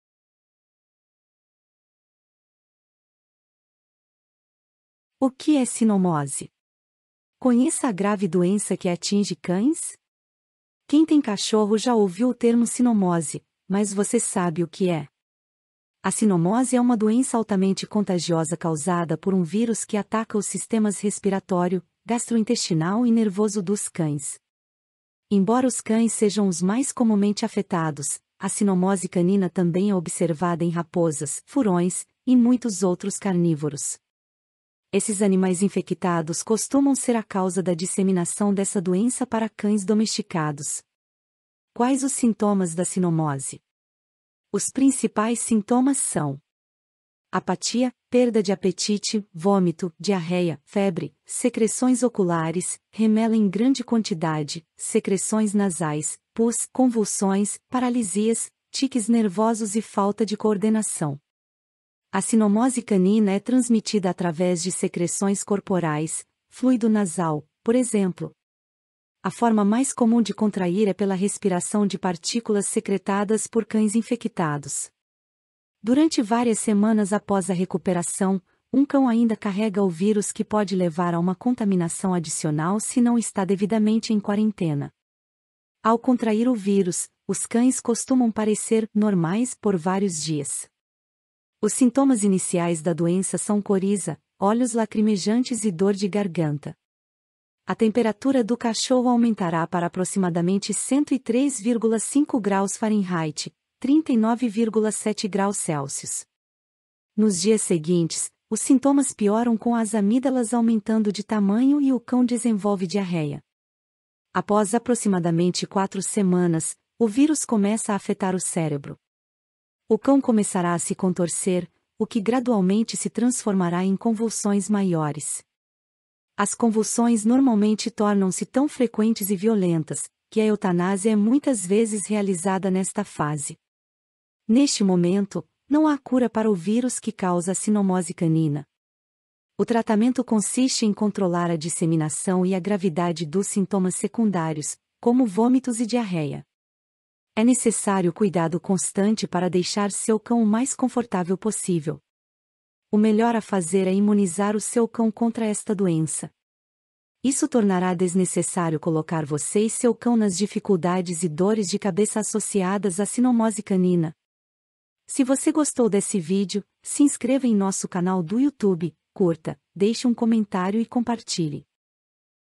O que é cinomose? Conheça a grave doença que atinge cães?
A cinomose é uma doença altamente contagiosa causada por um vírus que ataca os sistemas respiratório, gastrointestinal e nervoso dos cães.